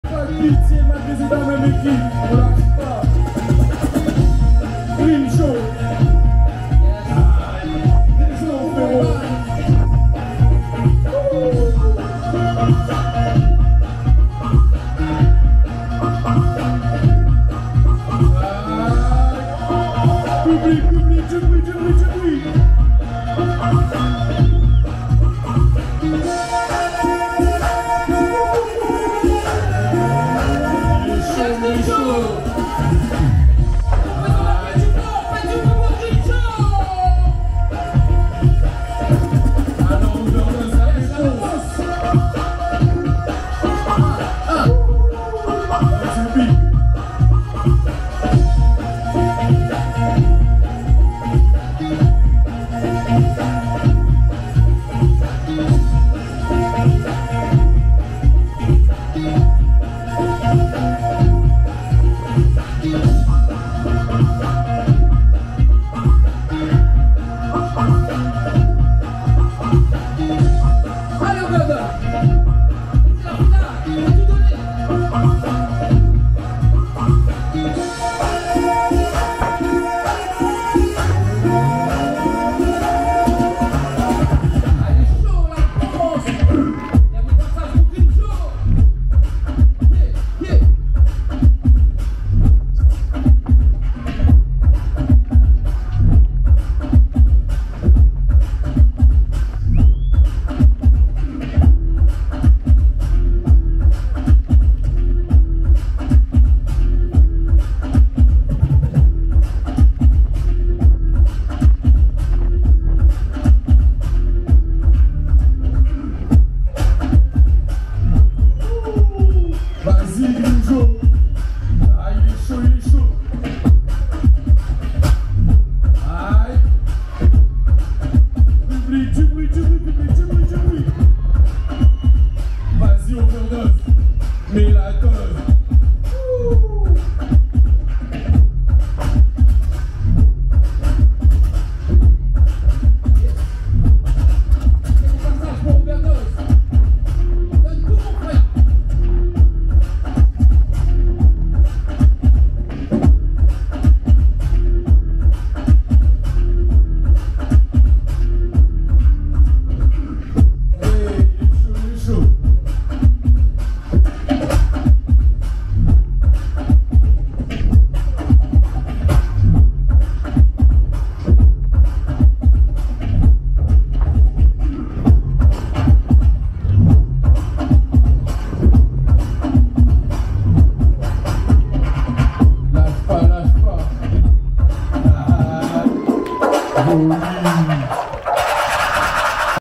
OVERDOSE vs GRIMJOW - Battle Terre2Jeux 2024
Battle Handi-Break
Final

Winner : Grimjow